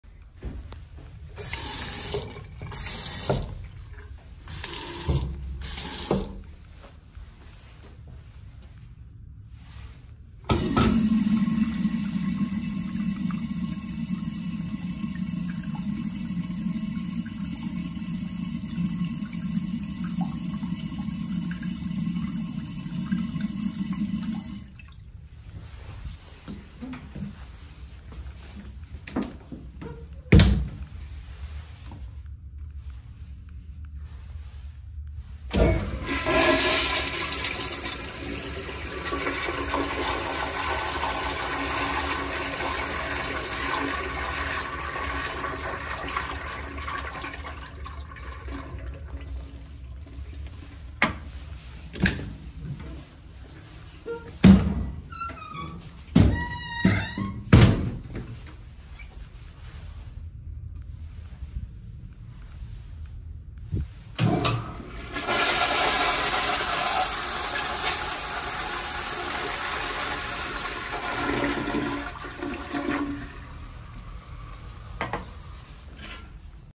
6,567. Crestmont Building Men’s Restroom
This restroom was filmed at the same building as the last 2 videos. The fixtures are: 1 1990’s AS Washbrook, 1 1990’s AS Madera, and 1 1990’s AS Cadet on flushometer. This restroom is nice!! Rating: 10/10